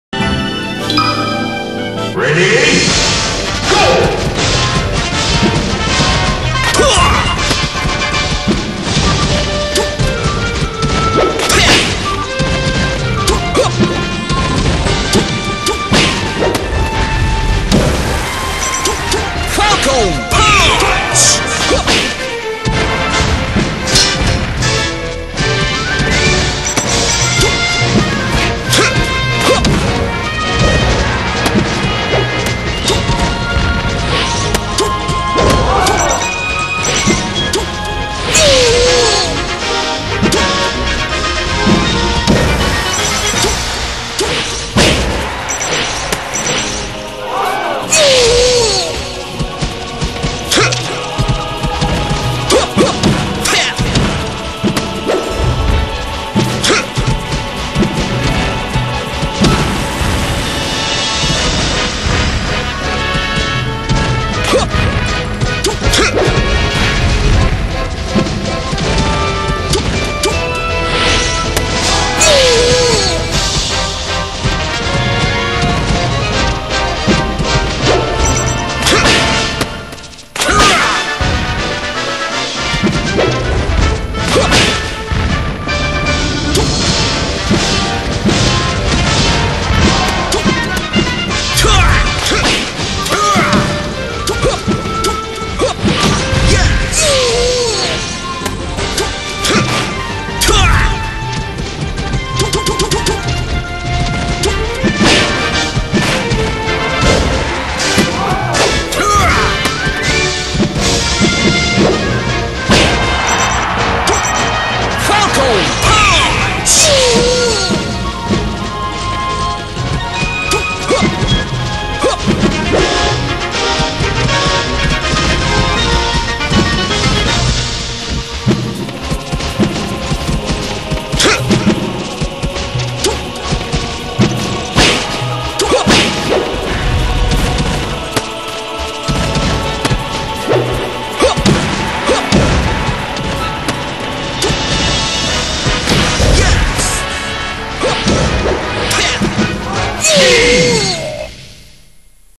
UVC - Marl (Cap. Falcon) VS Dyna DASH (Red Cap. Falcon)
A excelent and extreme fight between Marl sp of Ultra Violence Crew and Dyna DASH that outside also Ultra Violence Crew...

Awesome macth!!!